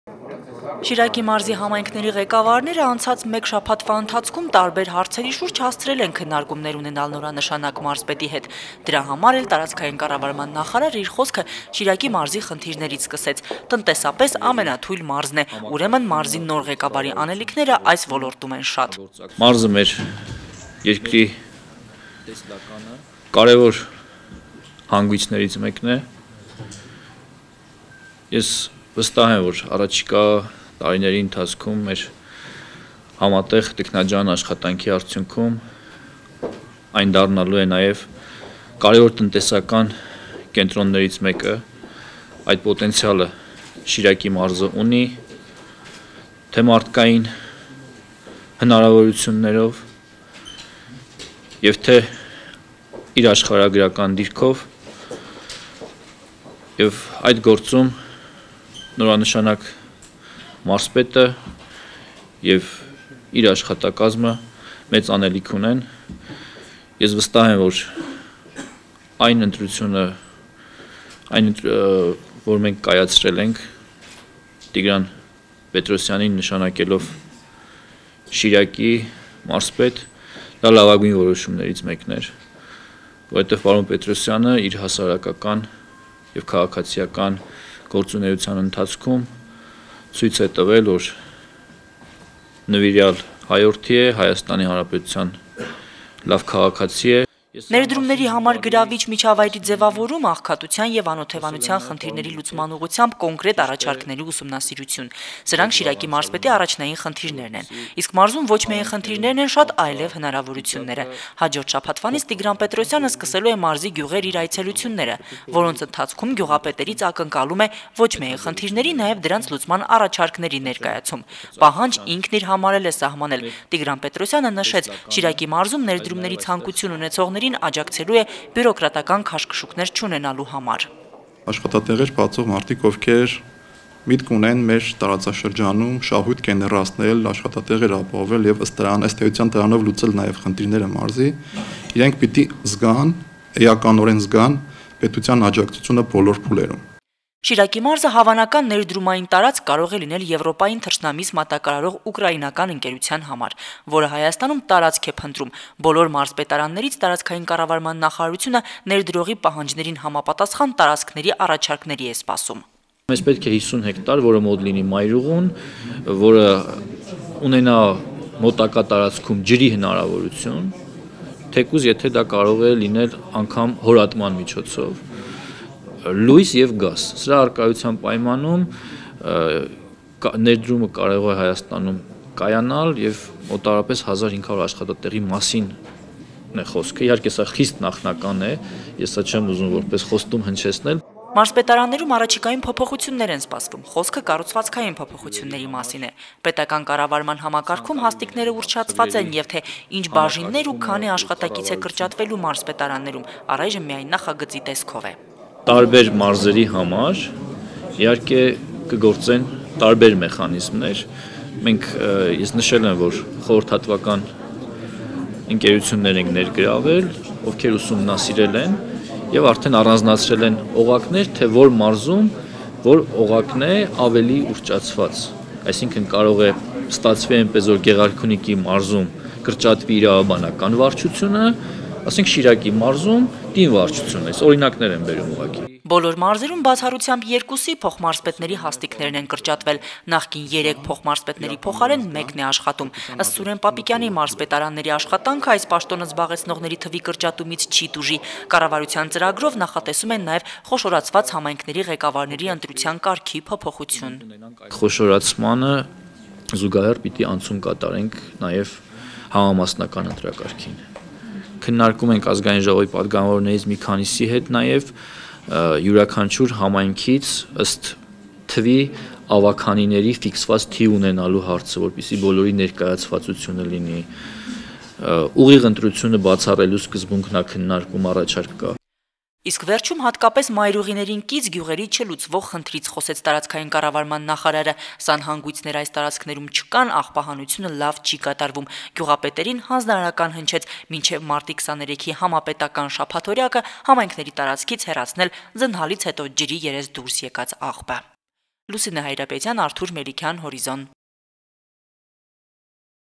Հորիզոն 13 02 2019
Տիգրան Պետրոսյանը 1 շաբաթ առաջ    ստանձնեց  Շիրակի մարզպետի պաշտոնը , իսկ  այսօր    Տարածքային կառավարման և ենթակառուցվածքների նախարար Սուրեն Պապիկյանը   Շիրակի մարզի համայնքապետերին պաշտոնապես  է  ներկայացրել   մարզի նորանշանակ ղեկավարին :  Մարզի ղեկավարի անձը կարևորել է հաշվի առնելով նրա հասարակական ակտիվ գործունեությունը, որի ընթացքում կուտակված փորձն, ըստ նախարարի, միայն կնպաստի Շիրակի մարզի արդյունավետ կառավարմանը : Իսկ մարզպետների լիազորությունները առաջիկայում կընդլայնվեն: Տեղական ինքնակառավարման մարմինների մասին օրենքի նախագծով նախատեսվում է ընդլայնել մարզպետների լիազորությունները գյուղատնտեսության ոլորտում: